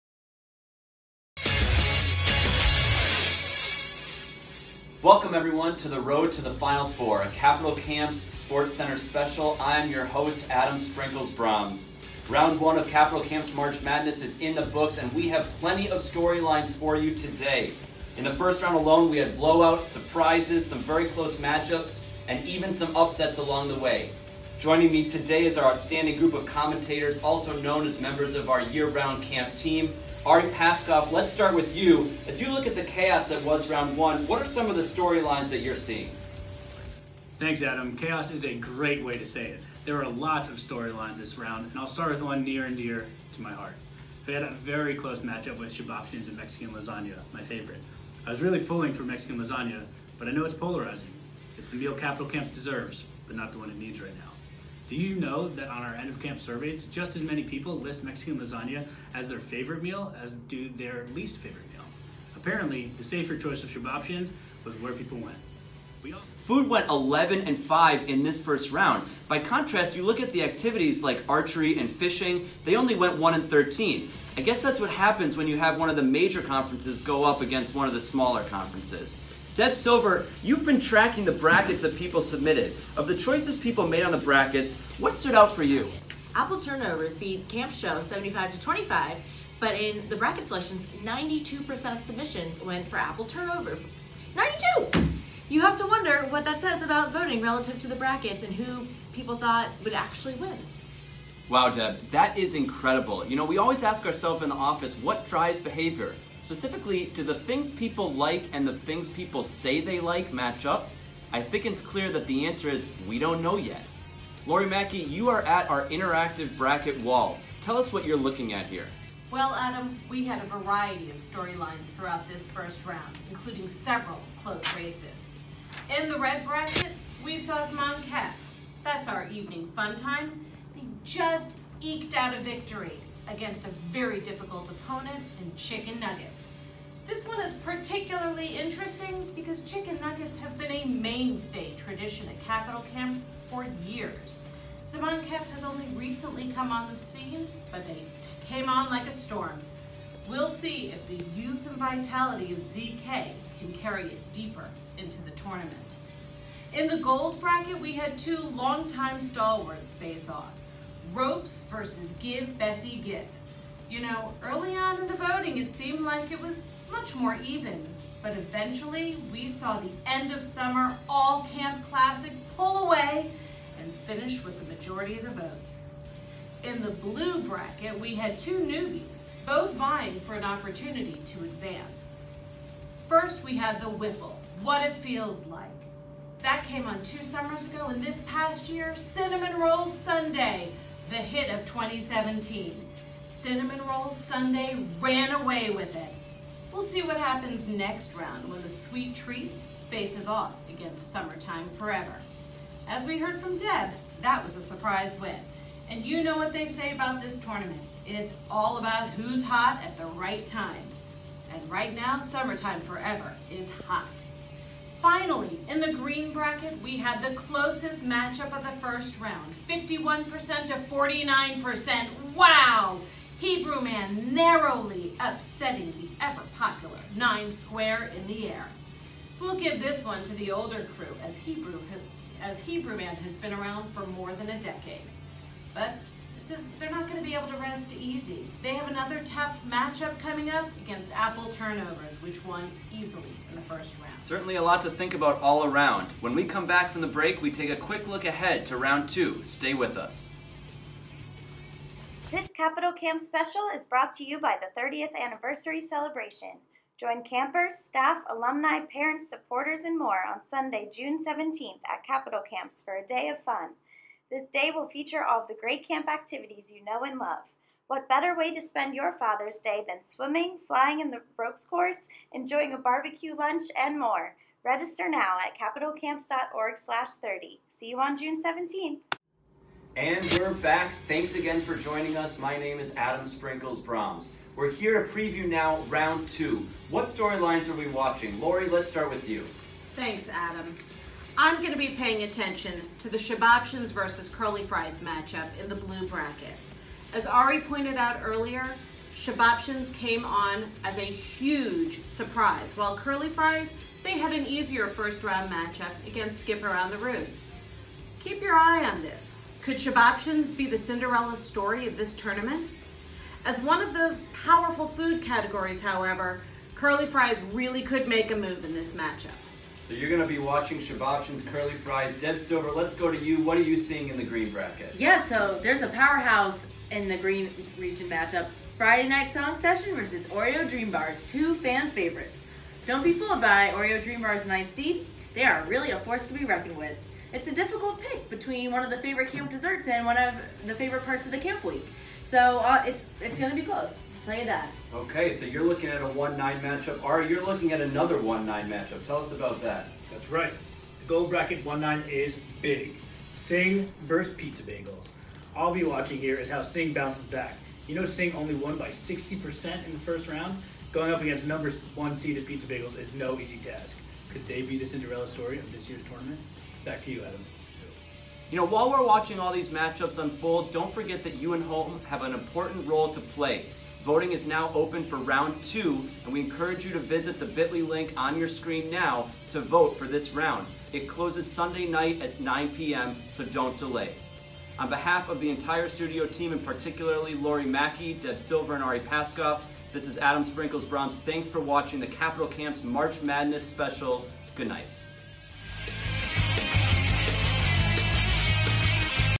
Capital Camps March Madness Round 1 Recap
Round 1 is in the books. Round 2 is here! Check out this video for a recap of Round 1, a preview of round 2, and don't forget to vote! for Round 2 voting!